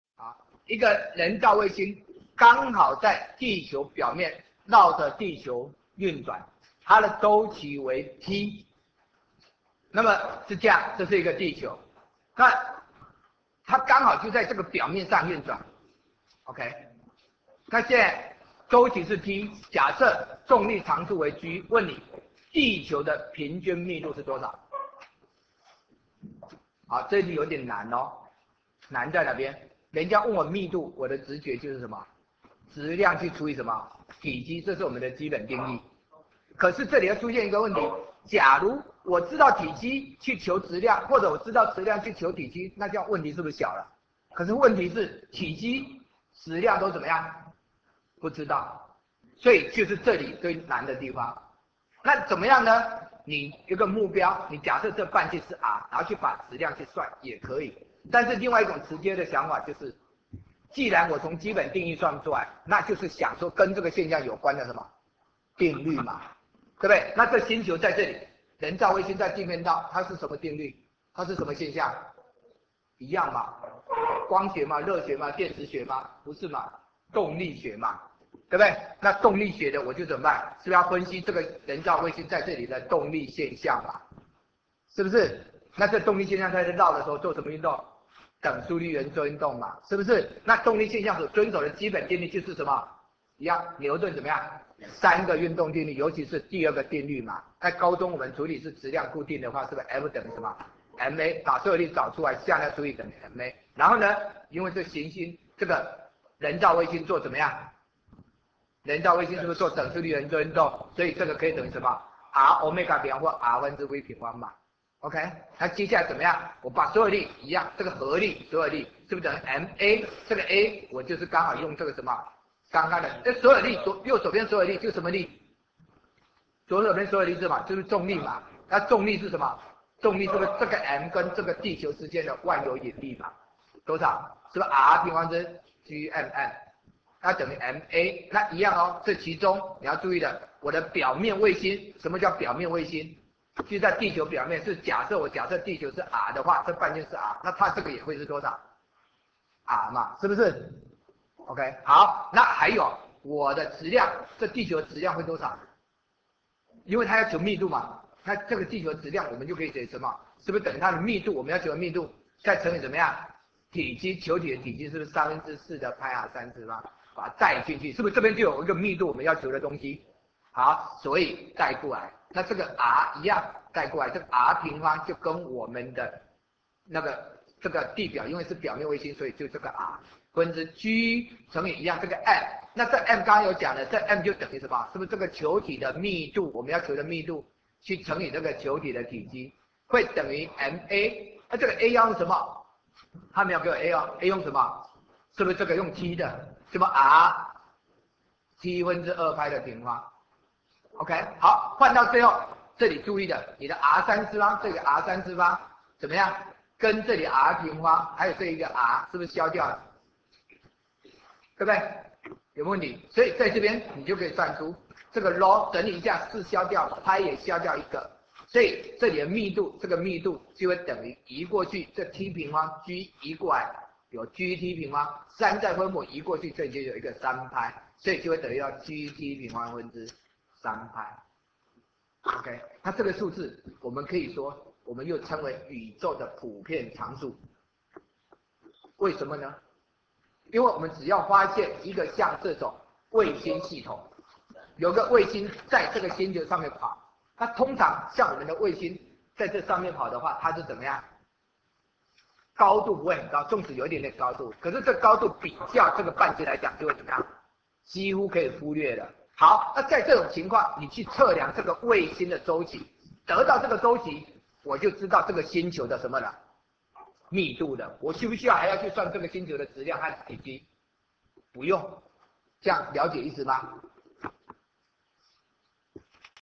[物理教學影片]範例:表面衛星
一人造衛星剛好在地球表面繞地球運轉,其週期為T。設重力常數為G,則地球的平均密度為何?【83南女】